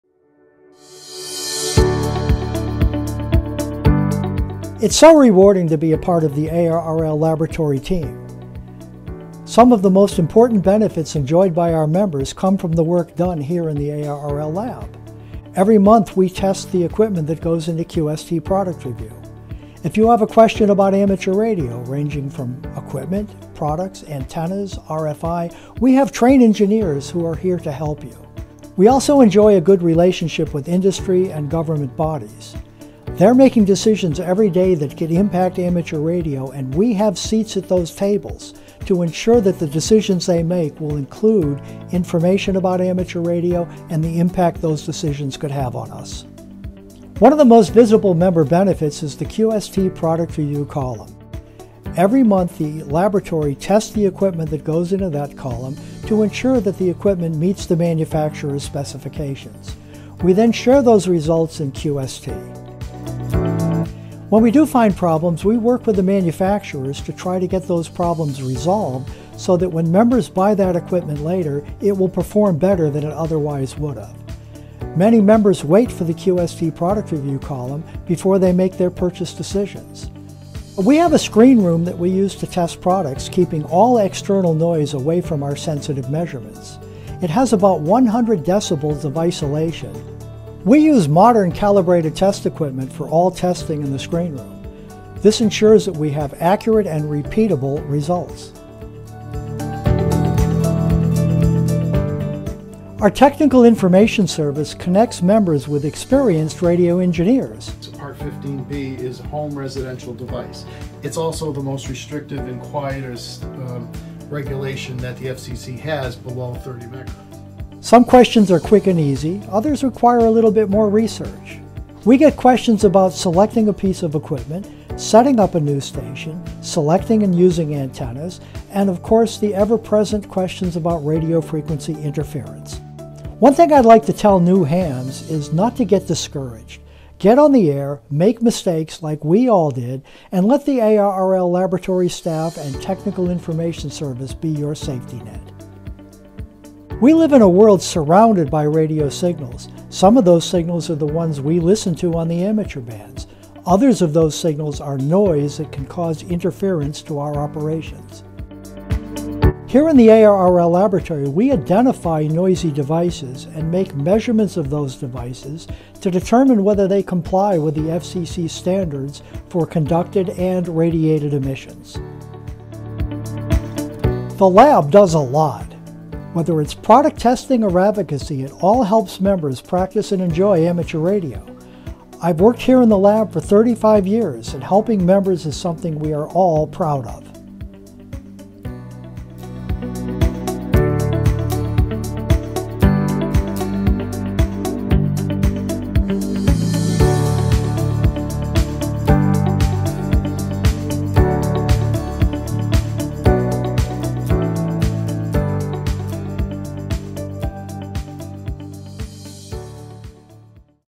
The ARRL Laboratory | providing technical services and support for ARRL members
The ARRL Laboratory completes some of the most important work supported for ARRL members and for the benefit of all radio amateurs. This introduction to the ARRL Lab, featuring Lab Manager Ed Hare, W1RFI and Paul Cianciolo, W1VLF, provides a summary of these popular membership services:

Equipment Testing for QST Product Review
Technical Information Service
Technical Advocacy